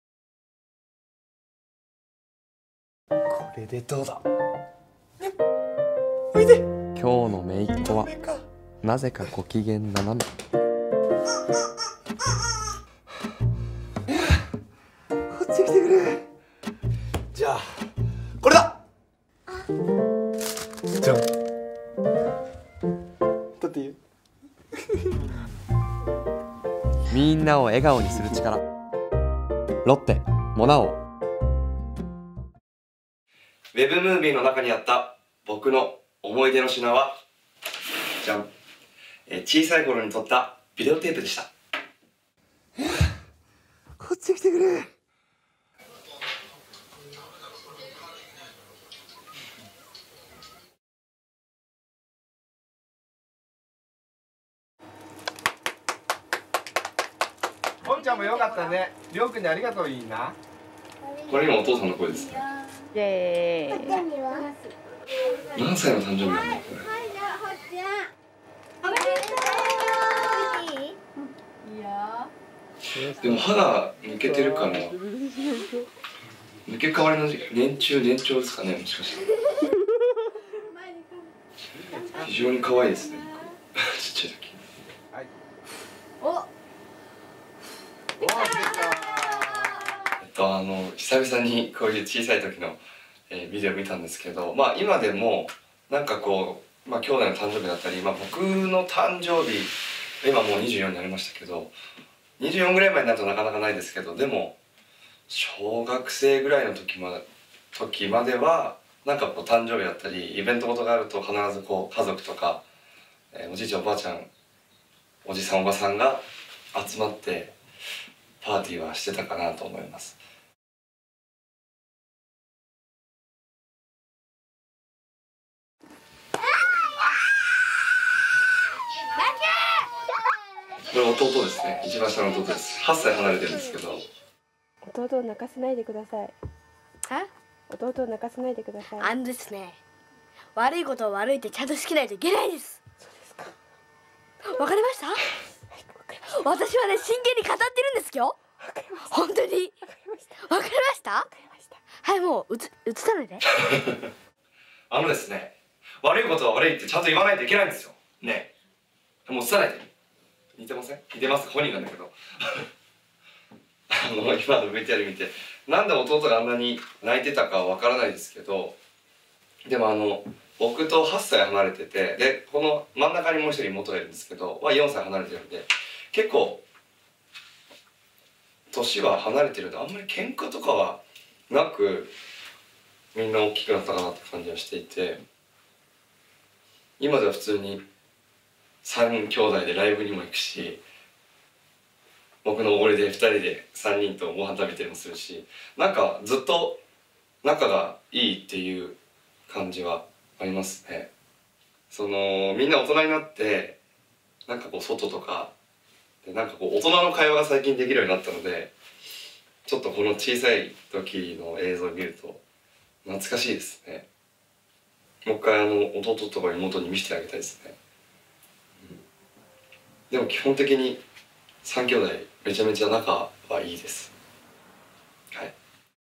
竹内涼真、幼少期の映像公開 ウェブムービーで猫耳コスプレも 「モナ王」ウェブムービー「家族をつなぐモナ王STORY」
俳優の竹内涼真さんが出演するロッテのアイス「モナ王」のウェブムービーが5月29日、公開された。インタビュー映像では、前歯が抜けた姿の誕生日会や、8歳離れた弟とけんかをするシーン、サッカーで華麗なフリーキックを決めるシーンといった竹内さんの幼少期を収めた貴重なホームビデオも登場する。また、ウェブムービーの本編では、竹内さんがひげを描き、耳を付けた猫に扮（ふん）した姿を披露している。

　ホームビデオを見ながらインタビューを受けた竹内さんは「歯が抜けているから（幼稚園の）年中か年長ですかね」と懐かしみながら、「非常に可愛いですね！　僕（笑い）」とにっこり。3兄弟で長男の竹内さんは「年齢が離れているせいか、兄弟とのけんかはほとんどなかった」といい「3兄弟でライブにも行くし、（竹内さんの）おごりで3人でご飯食べたりもする」というエピソードを語る。

　ウェブムービーは「家族をつなぐモナ王STORY」と題し、竹内さんが父、母、妹、めいっ子と一緒にアイスを食べる4編が楽しめる。さらに、竹内さんがサッカーでもらったメダルや、竹内さんの両親が撮影したビデオテープなど思い出が詰まった私物も登場している。本編、インタビューのほか、メーキング、竹内さんの24歳のバースデーをサプライズでお祝いした映像も公開されている。

　また、竹内さんのサインが入ったポスターが当たるキャンペーンも行われている。